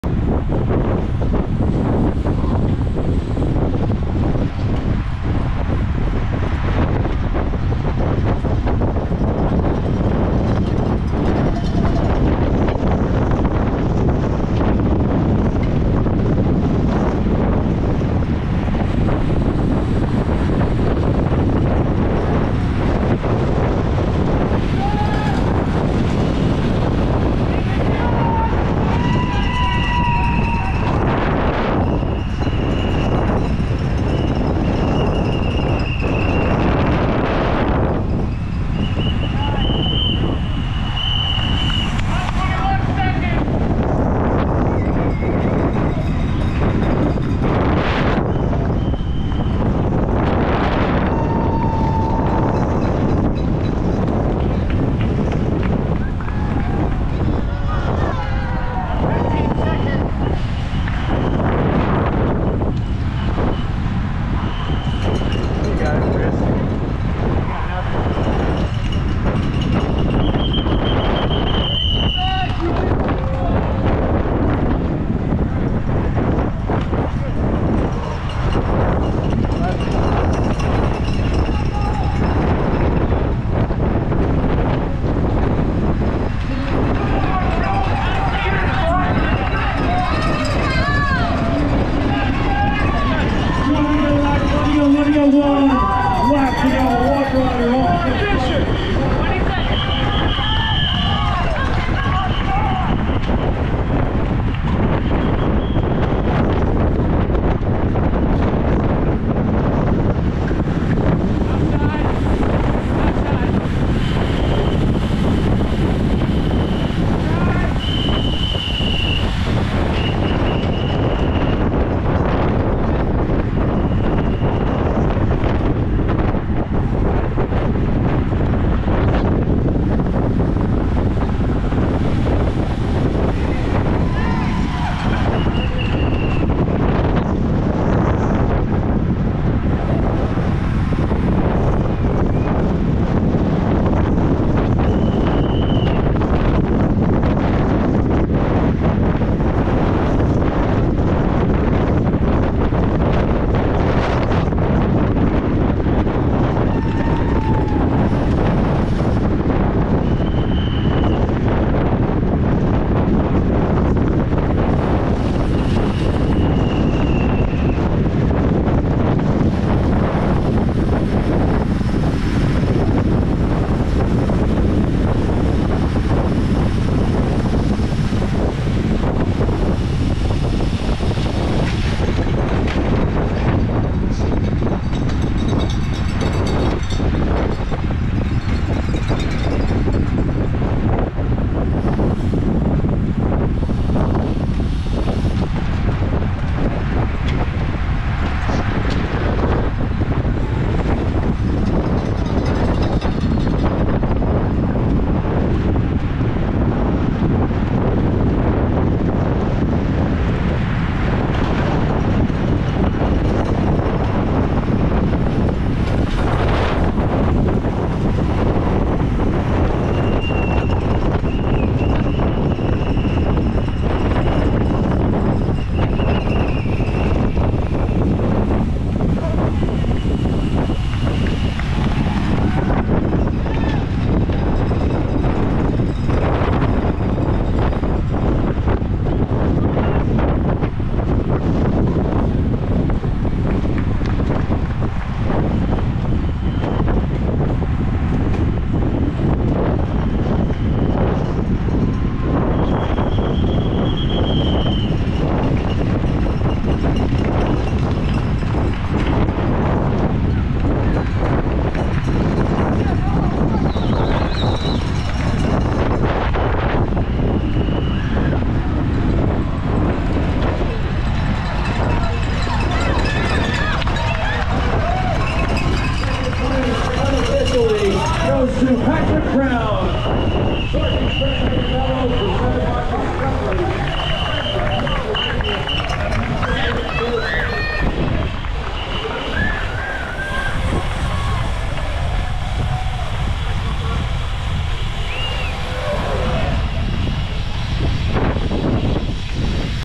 Bucks County Classic - Cat 2/3 - Last 1.5 laps
Jumped too soon, popped. Dropped.